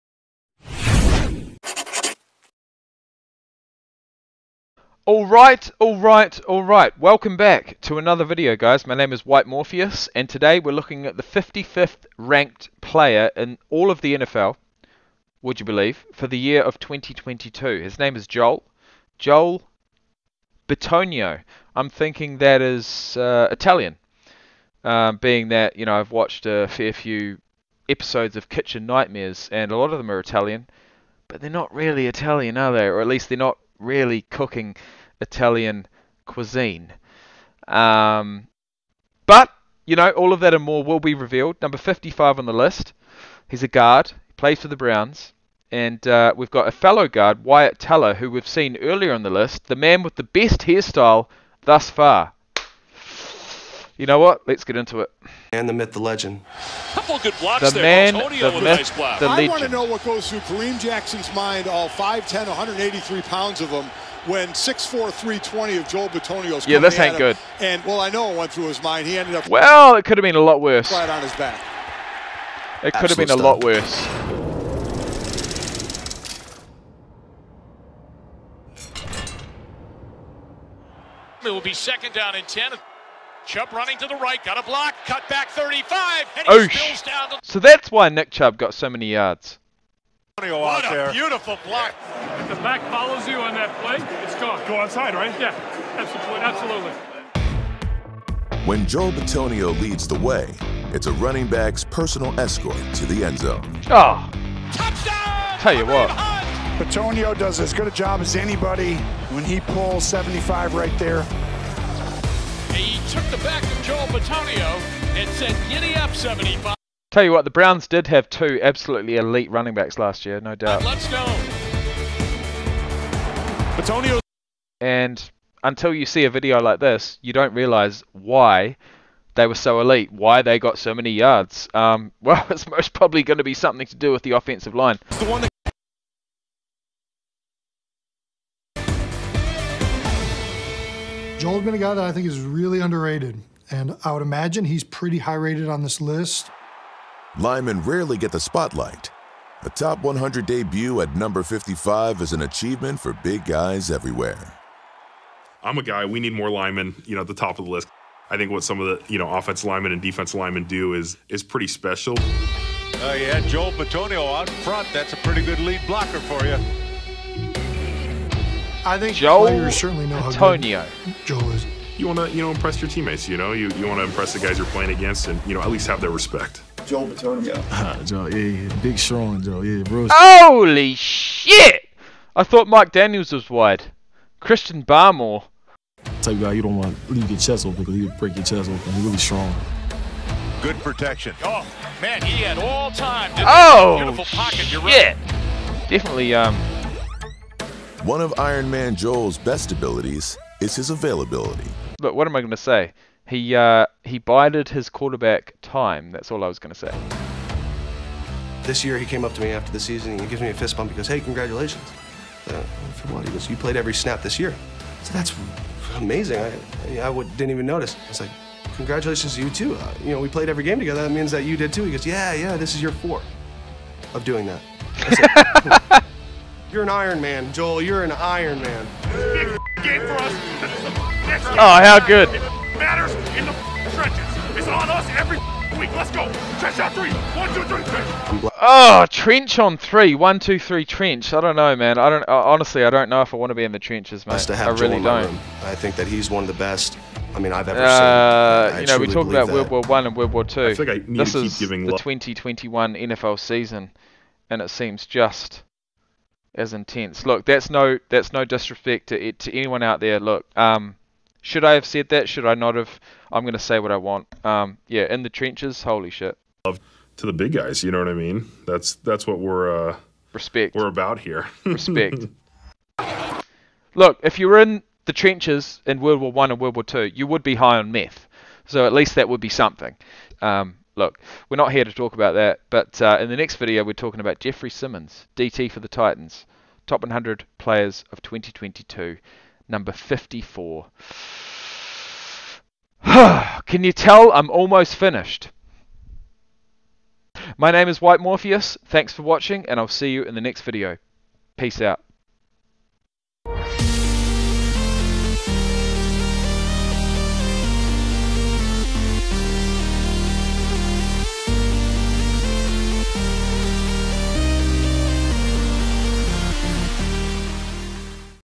Rugby Player Reacts to JOEL BITONIO (Cleveland Browns, G) #55 NFL Top 100 Players in 2022
After three consecutive years on the All-Pro second team, Bitonio finally broke through to become a first-team All-Pro in 2021. PFF graded him at fifth overall in run blocking the last two seasons, and he has committed only 13 penalties in the last six years. The Nevada product can hang his hat on being an integral part of Cleveland’s vaunted run offense and screen game, but the 2014 second-rounder has also allowed just three or fewer sacks in all but one season.